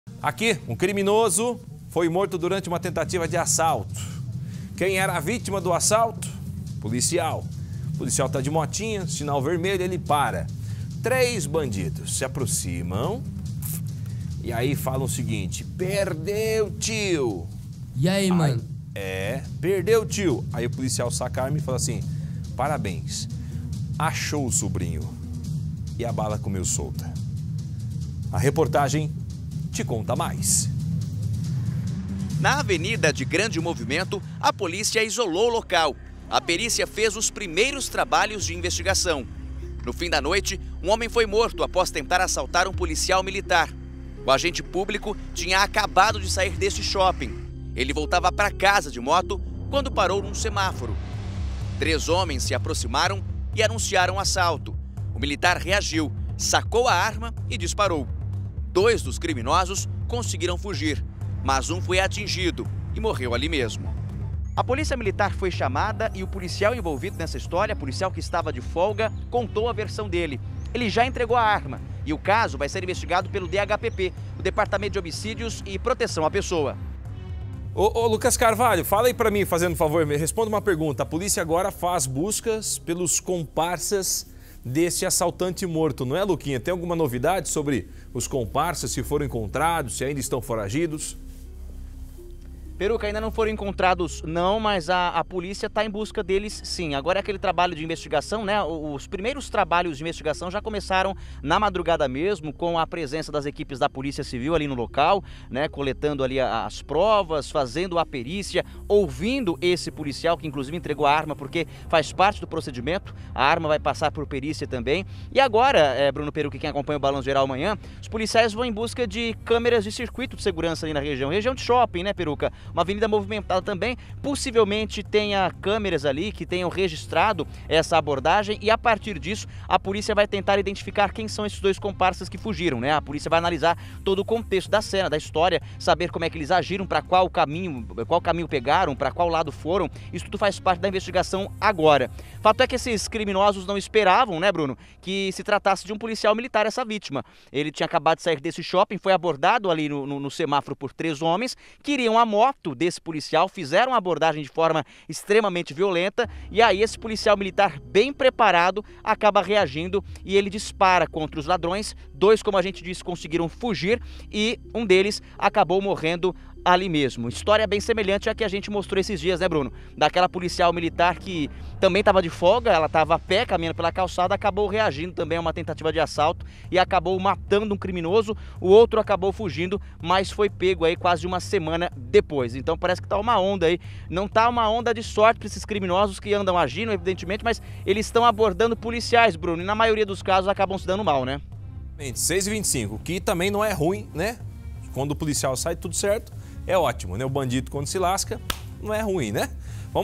PM de folga reage a assalto e mata bandido perto de shopping em SP
Três criminosos abordaram um policial militar que estava de moto e havia acabo de sair de um shopping, em São Paulo. O PM reagiu e matou um dos bandidos, já os outros dois assaltantes conseguiram fugir.